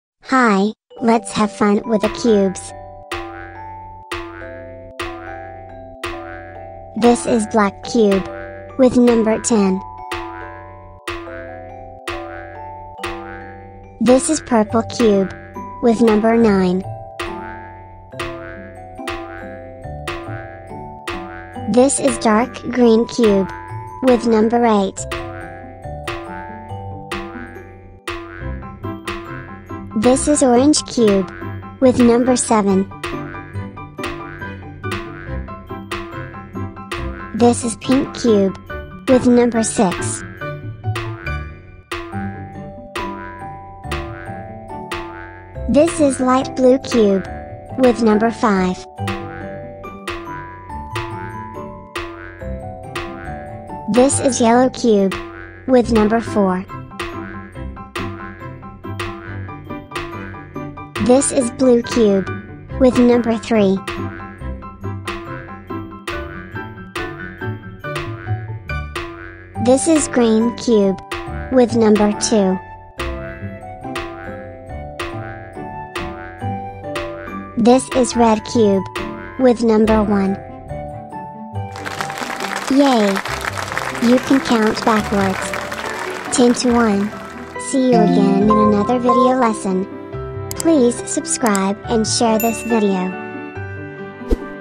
🌈 Color Cube Countdown! Learn Colors & Backward Numbers with Squishy Clay!
🎨 Let’s Count Backwards and Explore Colors! 🔢
Welcome to Zona Ilusi Anak, where learning is always colorful and fun! In this exciting episode, kids will discover brightly colored numbered clay cubes and count backward from 10 to 1 in a playful and visual way!

Your little one will enjoy watching squishy clay cubes in every color of the rainbow while learning how to count in reverse—an important early math skill made easy and fun!

✔️ Color recognition with rainbow clay cubes
✔️ Counting backwards from 10 to 1
✔️ Calm, engaging visuals for toddlers and preschoolers

🧠 Perfect for boosting:
• Number sense
• Color identification
• Memory & focus
• Early math skills